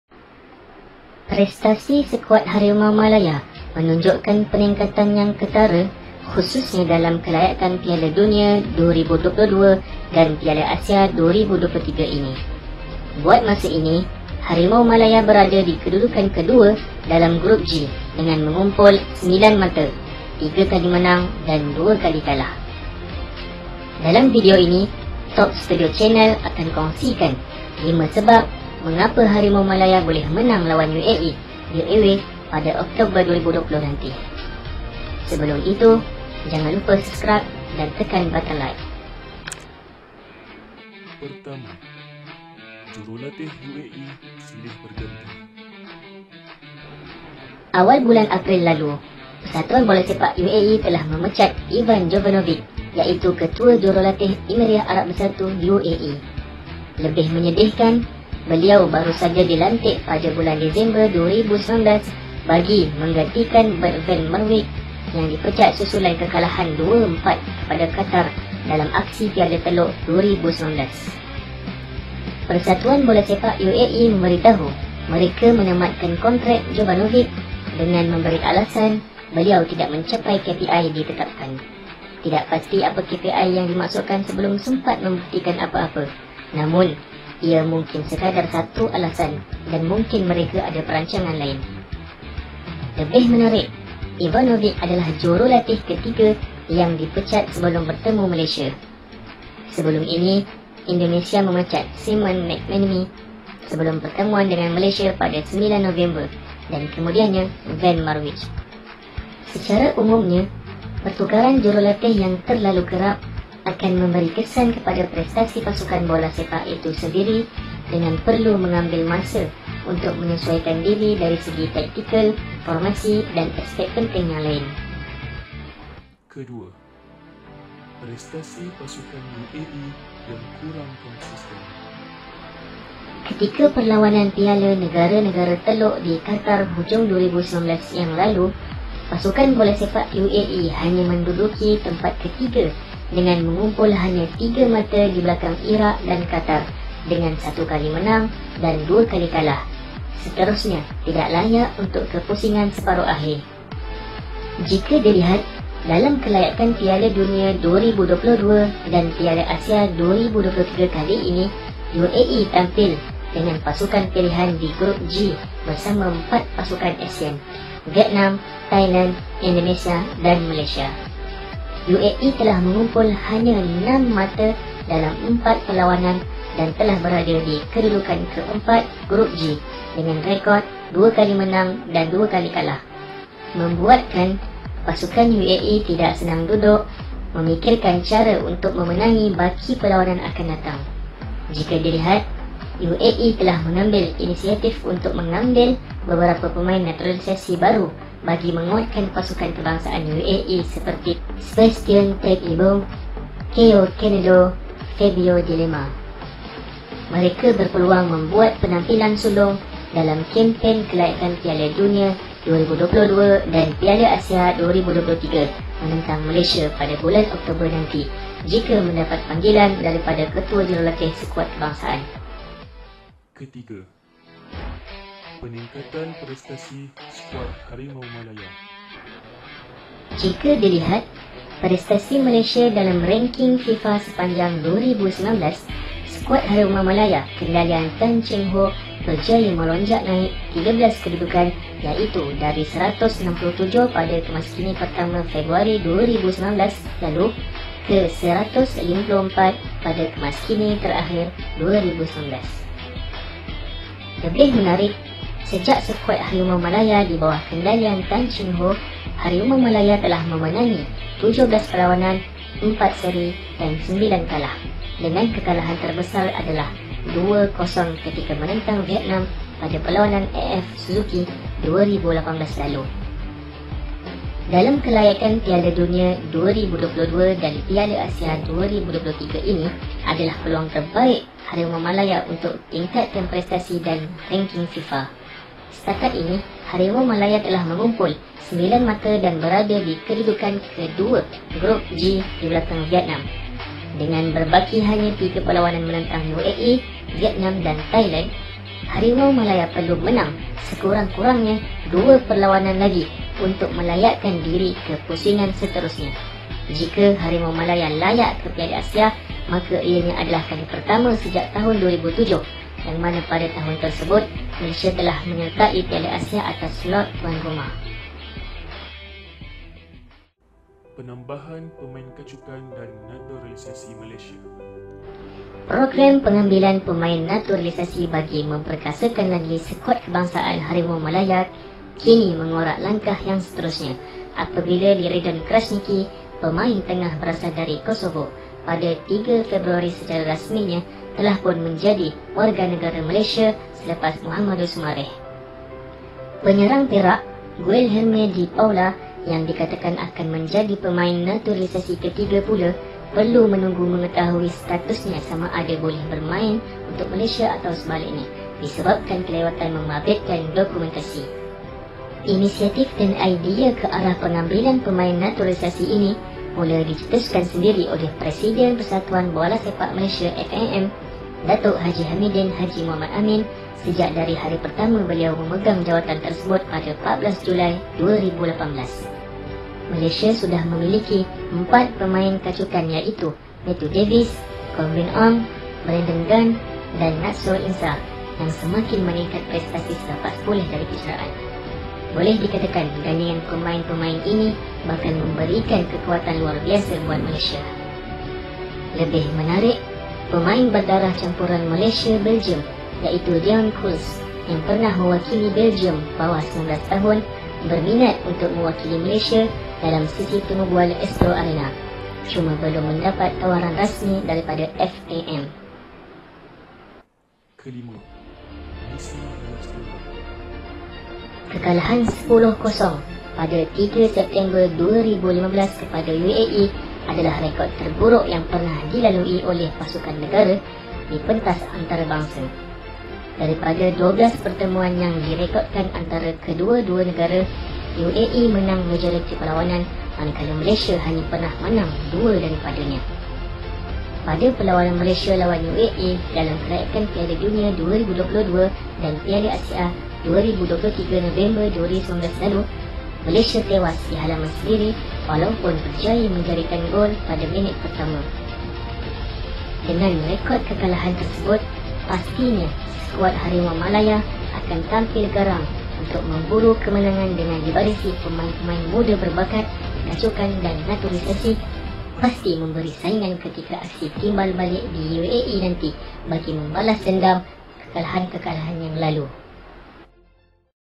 HARIMAU MALAYA boleh menang vs UAE di away Jun 2021 nanti? 5 sebab
nak tahu Harimau Malaya boleh pegi jauh ke tidak, boleh tonton video gameplay/tiki taka/teamwork semua perlawanan kelayakan WC/AQ Qualifiers yang admin dah buat ni:
Btw, selamat menonton
Boleh ke Malaysia menang ke atas UAE dalam perlawanan timbal balik yang diadakan di UAE dalam kelayakan Piala Dunia 2022/Piala Asia 2023 nanti? Sama-sama kita doakan Malaysia layak.